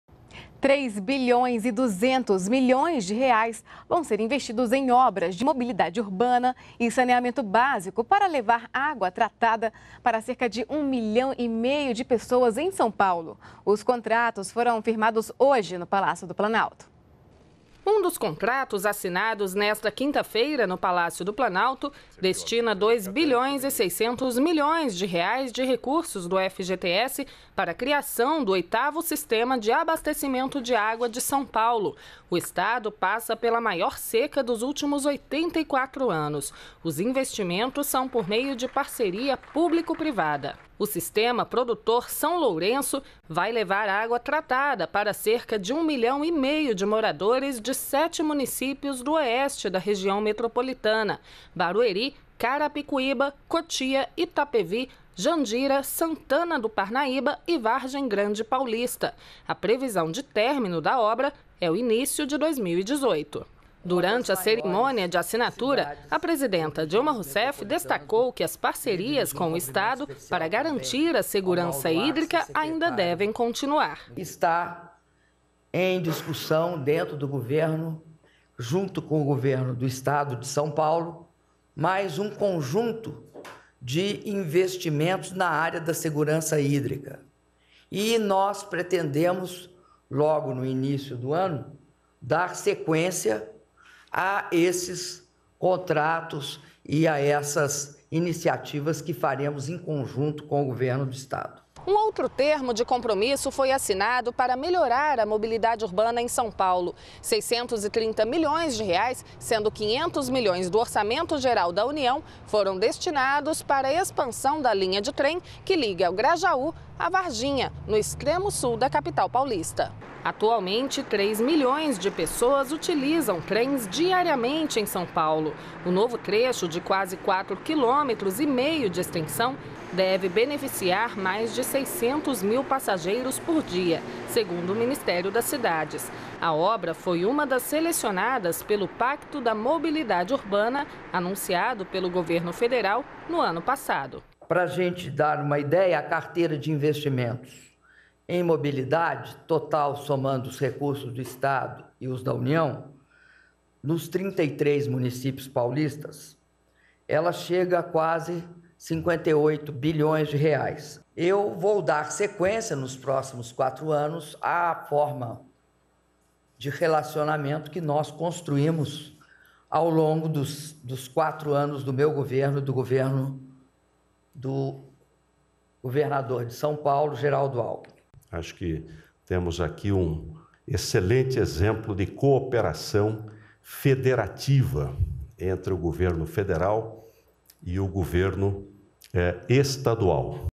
Assinado termo de compromisso para execução de obras no estado de São Paulo
NBR NOTÍCIAS - 04.12.14: O ministro das Cidades, Gilberto Magalhães Occhi, ao lado da presidenta Dilma Rousseff, participaram, nesta quinta-feira (4), da cerimônia de assinatura dos termos de compromisso para a execução de obras de mobilidade urbana e saneamento básico no estado de São Paulo. O investimento será de R$ 3,3 bilhões, sendo R$ 1,4 bilhão do Orçamento Geral da União (OGU) e R$ 1,8 bilhão de contrapartida estadual.